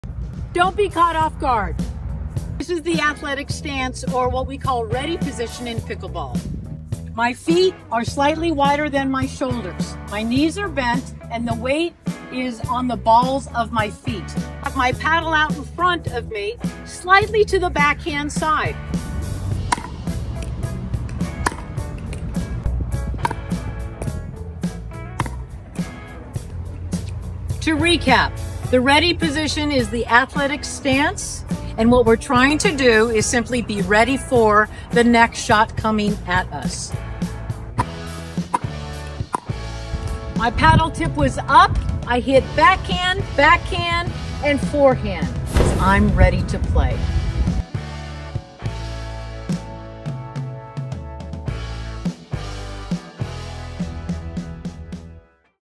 The Most Important Pickleball Position You Need to Know
@ZSistersPickleball - Learn the essential pickleball ready position! This short video demonstrates the correct athletic stance, foot placement, knee bend, and paddle positioning to ensure you're always prepared for the next shot.

All products we mentioned in this video are products or services we use ourselves, love and recommend. And they can help YOU!

⏰ Timecodes ⏰

⏰ Timecodes ⏰
00:00 Introduction
00:11 Athletic stance
00:22 Paddle in front towards backhand side
00:33 Be ready